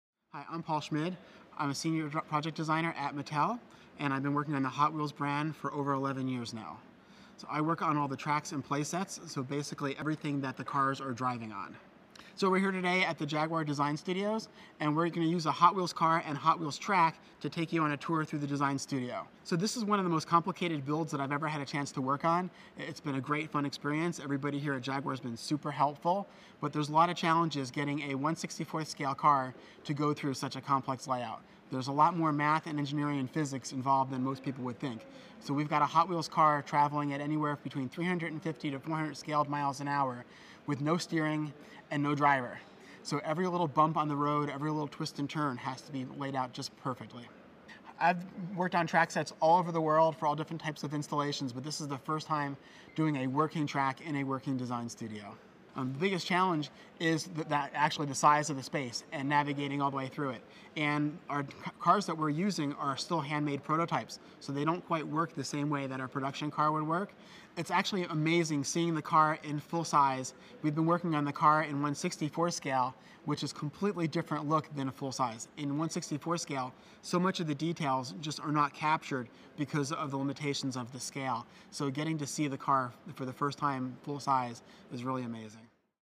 The New Jaguar F-TYPE - Paul Schmid, Senior Project Designer, Hot Wheels®
"Purposeful, beautiful and timeless: the new Jaguar F-Type looks more dramatic than ever and embodies Jaguar design DNA in its purest form.

The perfect proportions are instantly recognisable, but the new F-Type looks even more focused and assertive, and references the beautiful flowing forms from Jaguar's rich sports car heritage.

Super-slim pixel LED headlights with subtly updated 'Calligraphy' J signature daytime running lights and sweeping direction indicators blend perfectly into the 'liquid metal' surfacing of the new clamshell bonnet. The new front bumpers and subtly enlarged grille deliver even more visual impact and presence.

The rear haunches enhance the F-Type's inherently dramatic, purposeful form, and sweep downwards to the slender rear LED lights with 'Chicane' signature introduced by the all-electric Jaguar I-PACE Performance SUV.

Inside, the driver-focused '1+1' cabin envelops the occupants with luxurious materials such as Sienna Tan Windsor Leather, Suedecloth, Alcantara, and satin-finish Noble Chrome, while Jaguar's craftsmanship is embodied in beautiful details such as monogram stitching on the seats and door trim. These touches blend seamlessly with the new 12.3-inch reconfigurable HD TFT instrument cluster and Touch Pro infotainment system.

Form follows function, and one of a Jaguar's functions is to look beautiful. Evolved from its predecessor's World Car Design-winning form, the new F-Type's breath-taking looks are characterised by the super-slim Pixel LED headlights, which enhance the car's purposeful, confident stance."